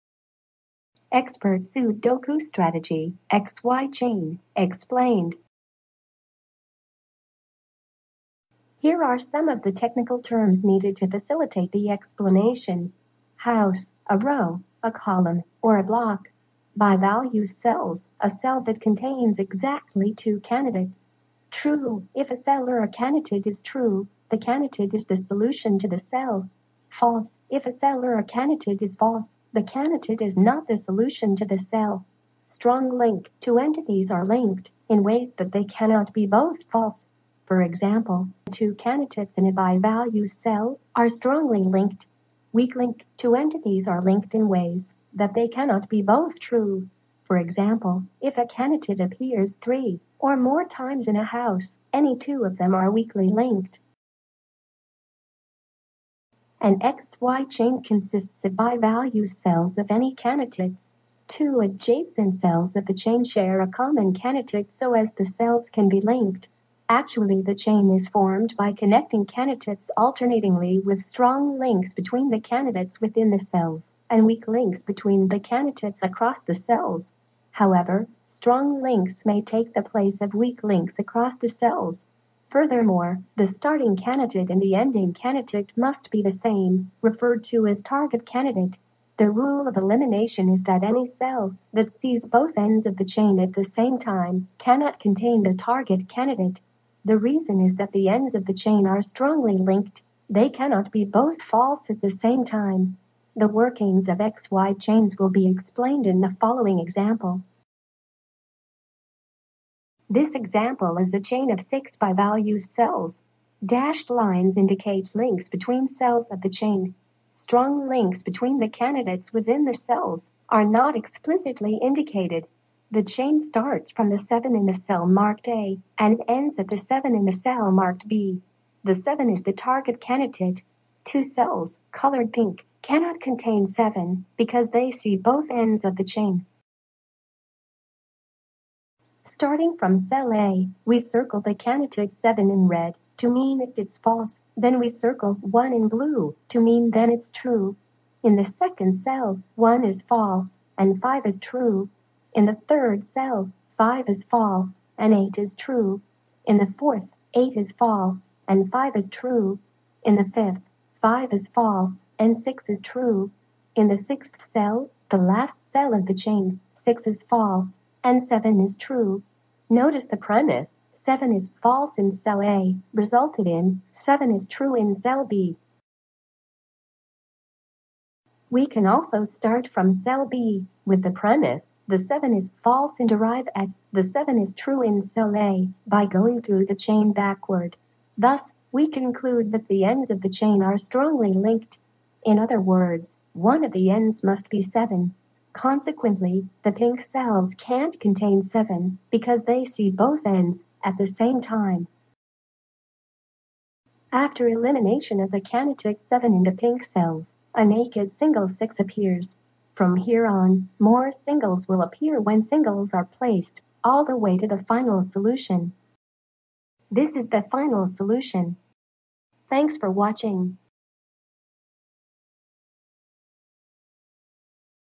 Expert Sudoku Strategy XY-Chain Explained
A strategy all Sudoku experts must know.